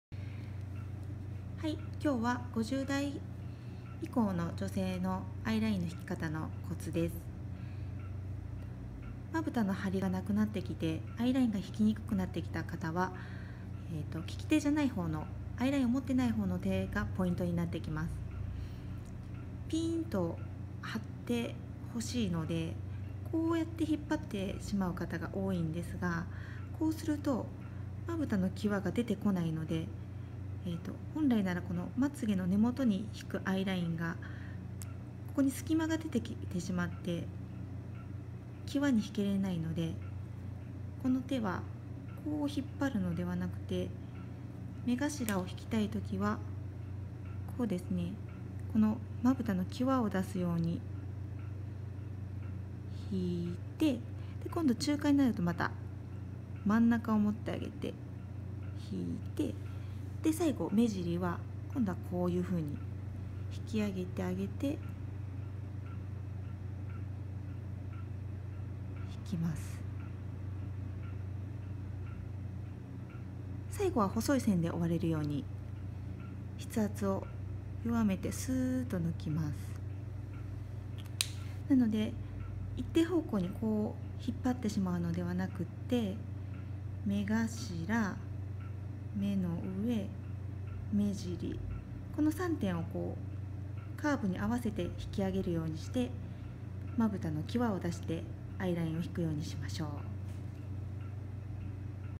【アイラインを上手に描くコツ】 広島県福山市 深夜26時まで営業 内海美容室
まぶたのハリがなくなり、アイラインがひきにくくなってきた大人の女性の為のアイラインのコツを動画でご紹介。

内海美容室ではマンツーマンのメイクレッスンを行っています。メイクが苦手な方ご予約ください。

広島県福山市 深夜26時まで営業ヘアサロン 内海美容室